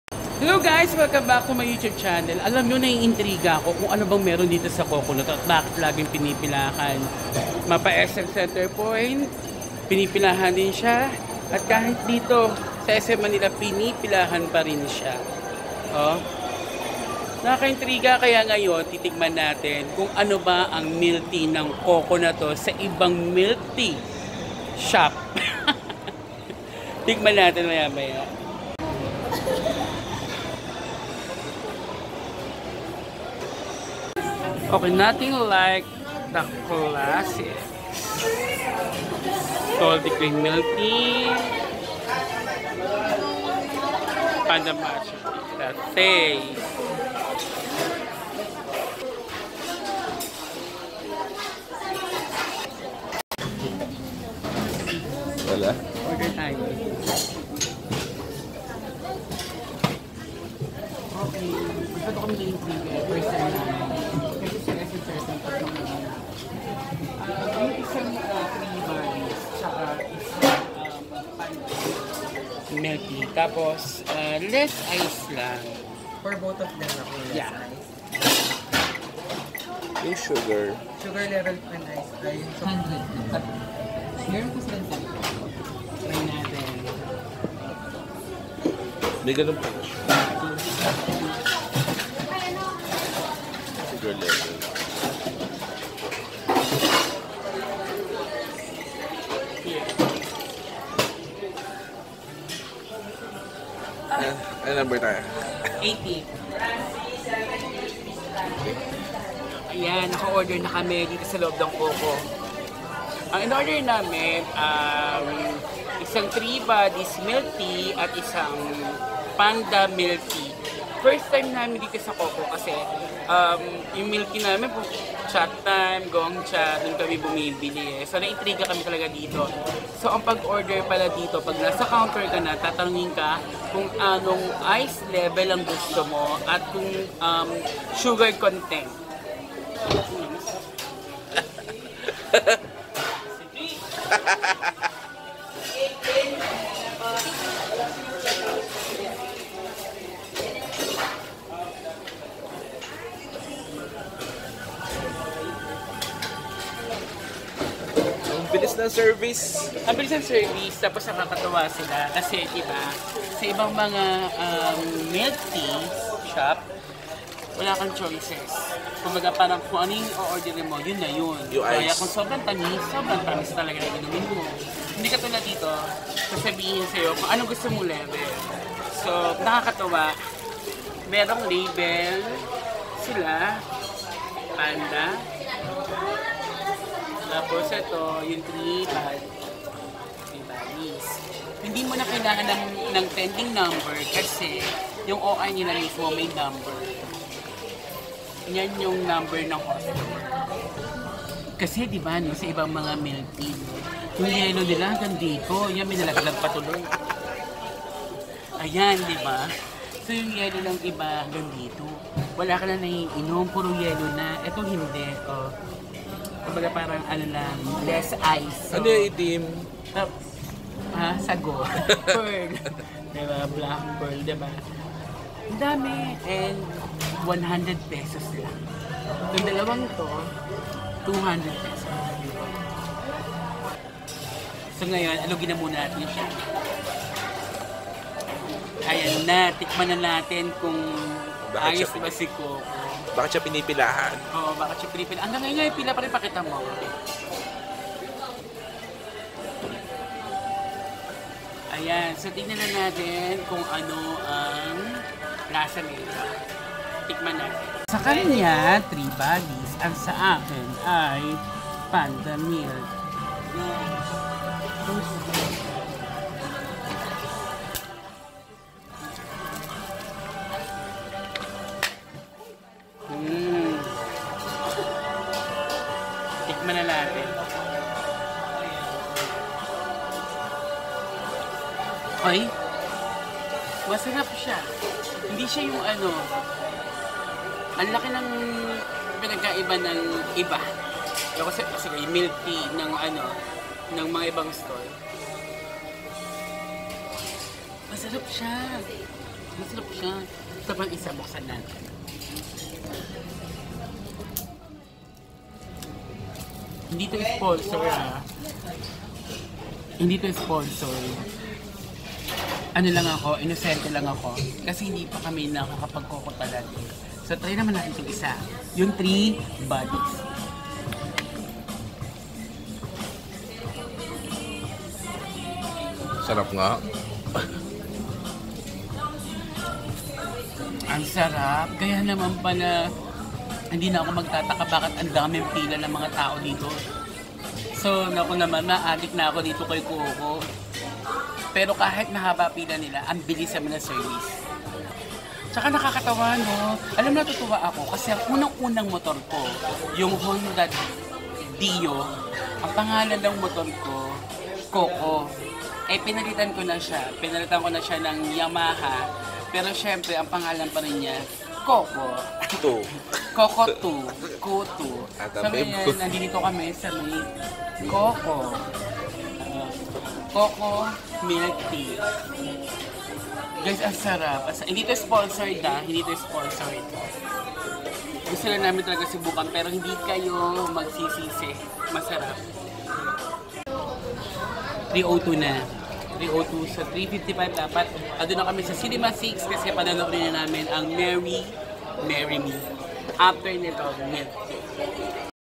COCO MILK TEA - REVIEW
Ever get curious seeing consistent long lines in every Coco Milk Tea stalls in SM Malls? May Promo ba? Ano pakulo nila? Bakit ang haba haba ng pila nila. samantalang sa ibang milk tea stores halos wala? Alamin po sa video coverage na to kung bakit!